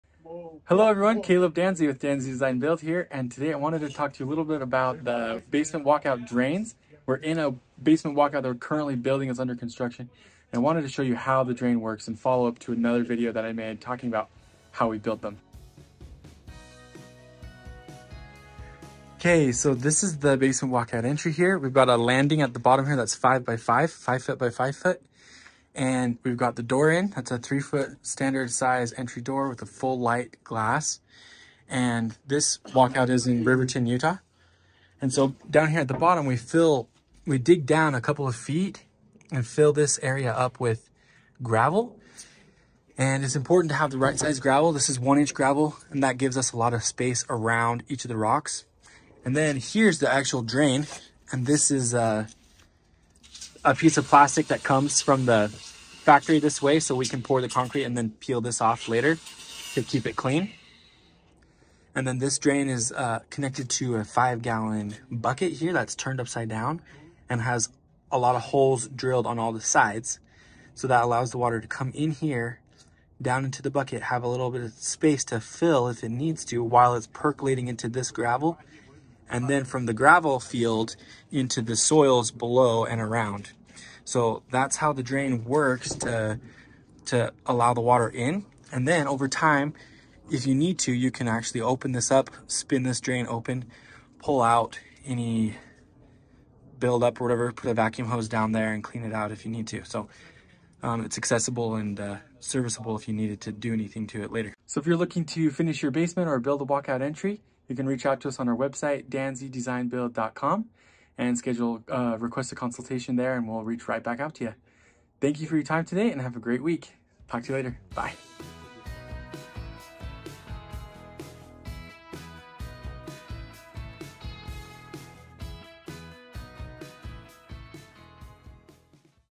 Explained: How Basement Walkout Drains Work
A basement drain is essential to your house. It helps prevent flooding and other problems. Learn more about them here!